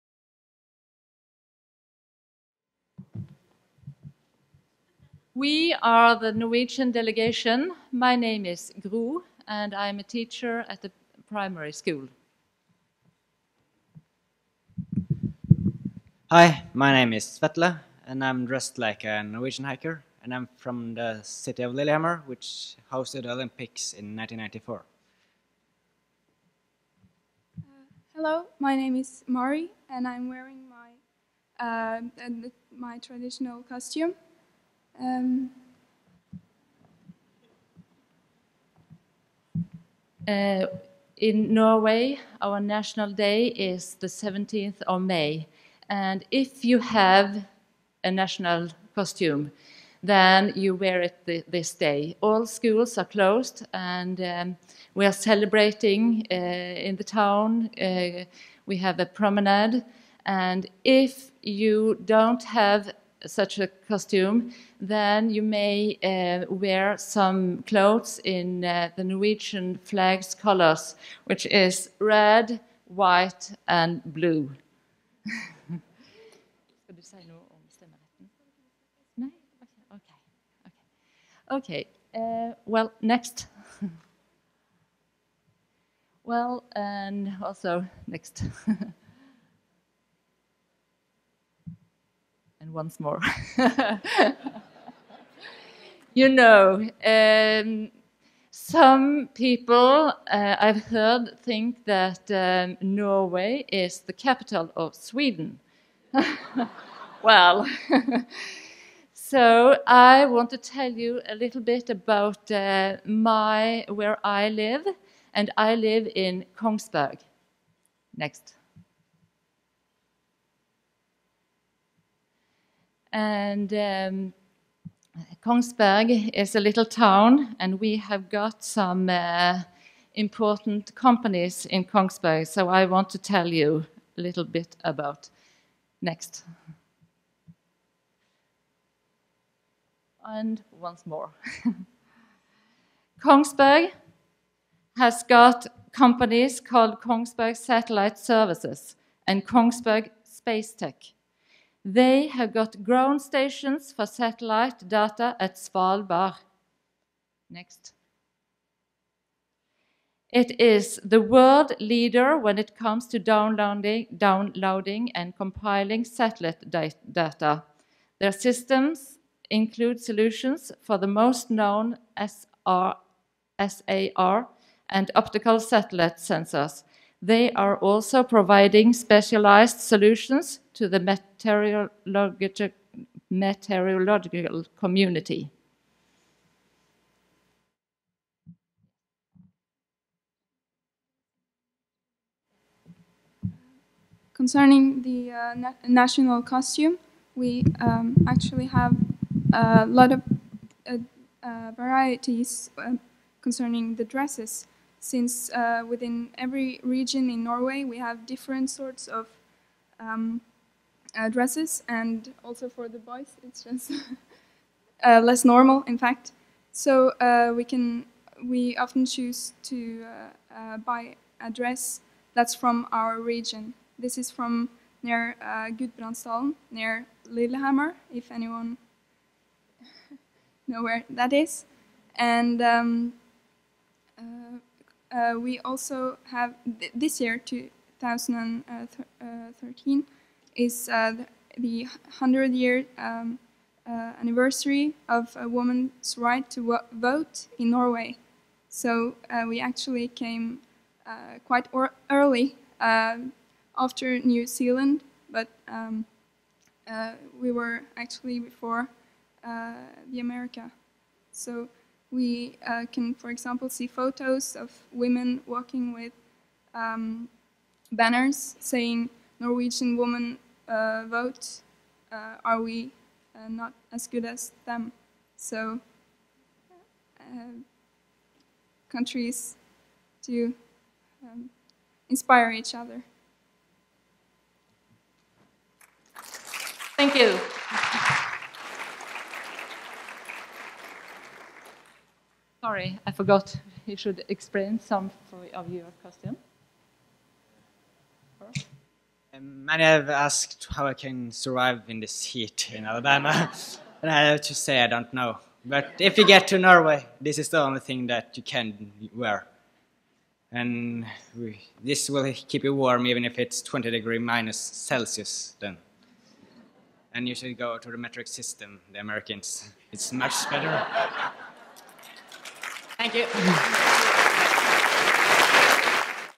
Norway - 2013 International Space Camp Opening Ceremony
Educator Gro Jore and students Vetle Brekmo and Mari Martinussen introduce their home country of Norway at the 2013 International Space Camp Opening Ceremony.